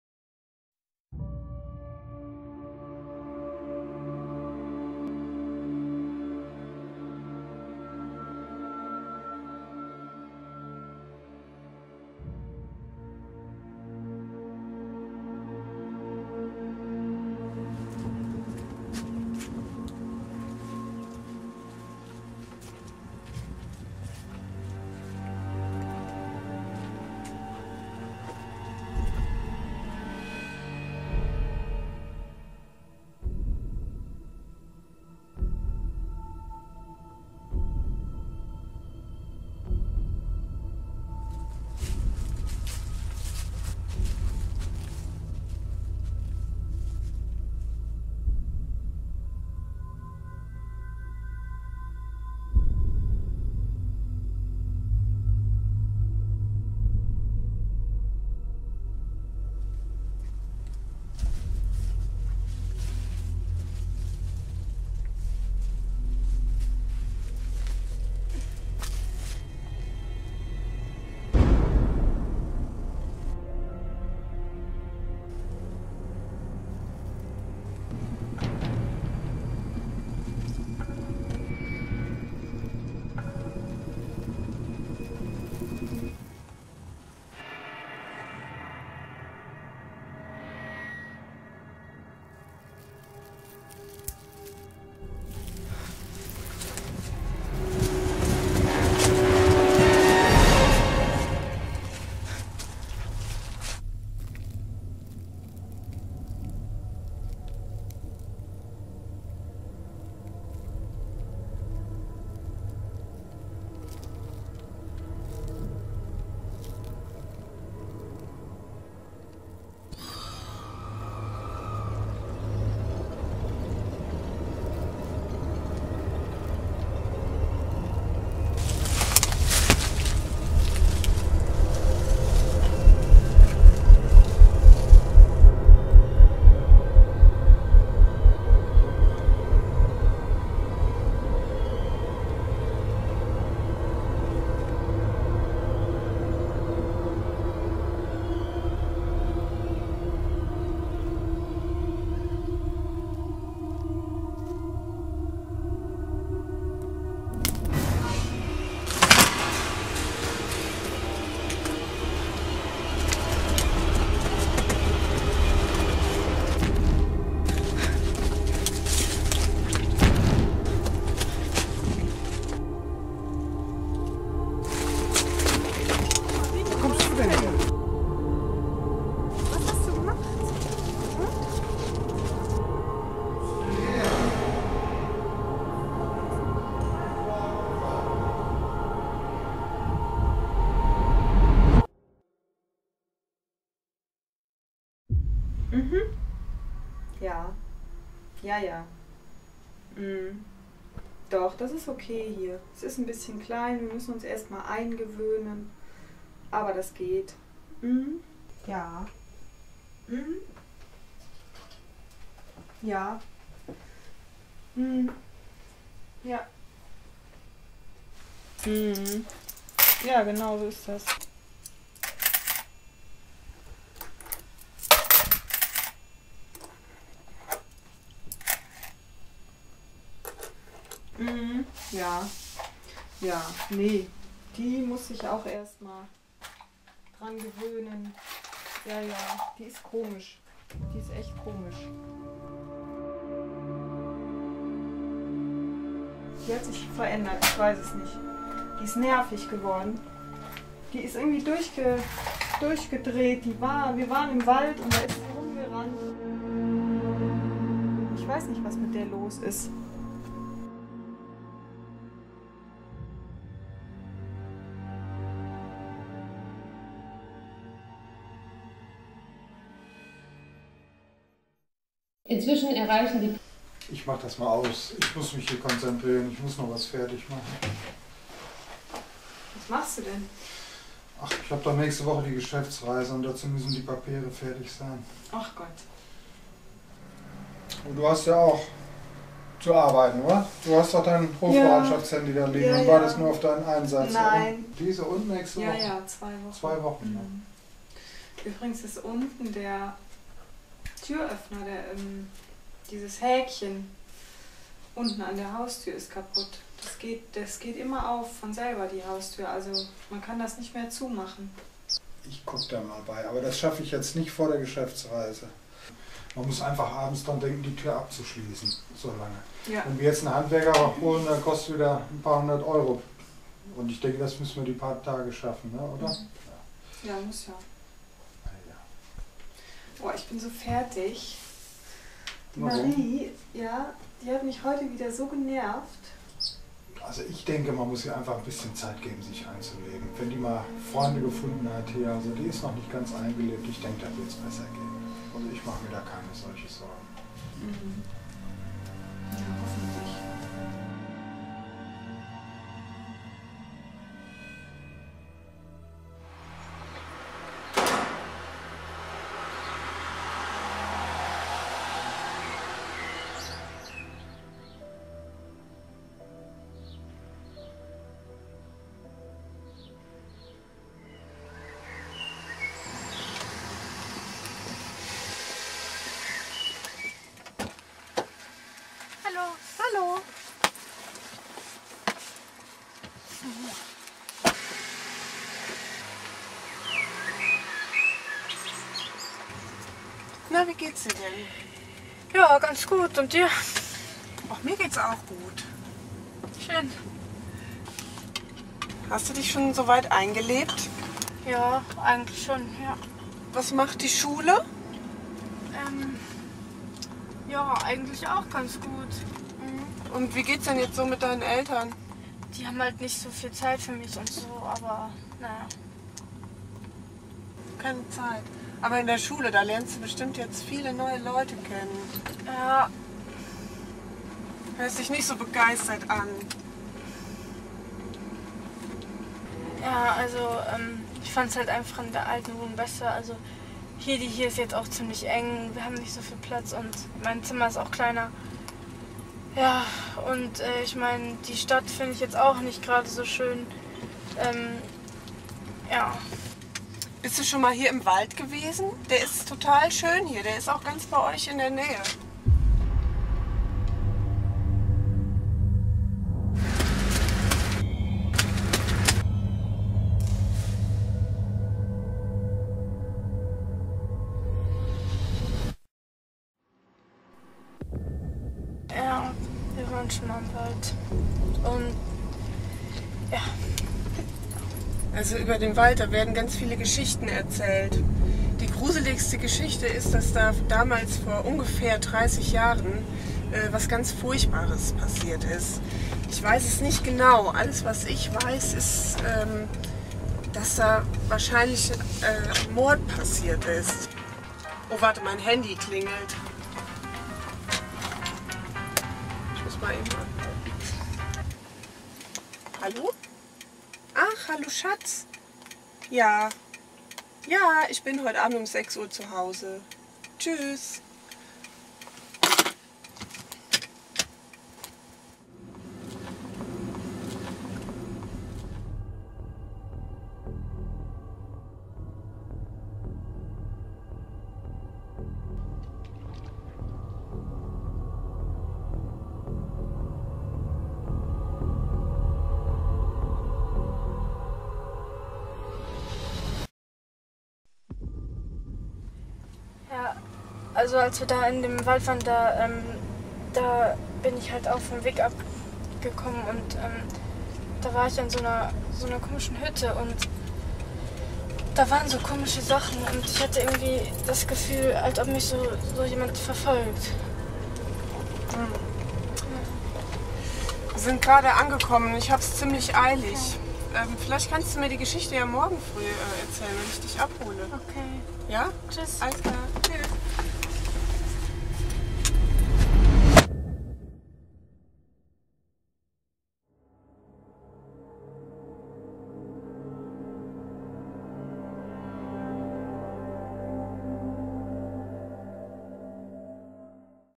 Das Haus im Wald - Teil 3
Der dritte Teil unseres Filmprojektes "Das Haus im Wald"
Hauptrolle: Chiara Löhr
Regie: Roman K.